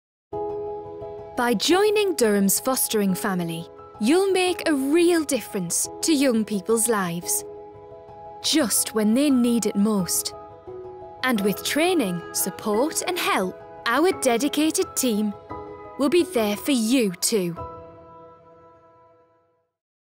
Durham Fostering - TV Advert | Honeypot Creative - Video Production in Newcastle
The Fostering Team at DCC take their responsibility to find and bring in the very best carers, for the many children they help to rehome, extremely seriously. As part of an on-going partnership, we are helping them to find and those couples and families most able to offer a new start or provide cover for children who need the stability and care.